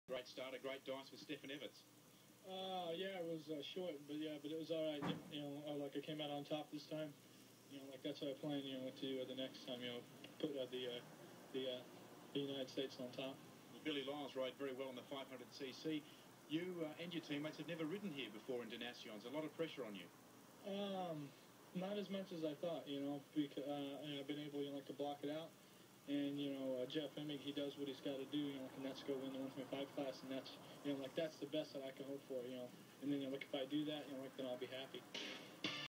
Young Jeff Emig
Early days of a young Jeff Emig!  Come a long way since this interview after a Moto race at the 1992 Des Nations at Manjimup WA Australia!  Pretty raw then... Now of course after a great career now commentates all the AMA Motocross and Supercross evens on SPEED!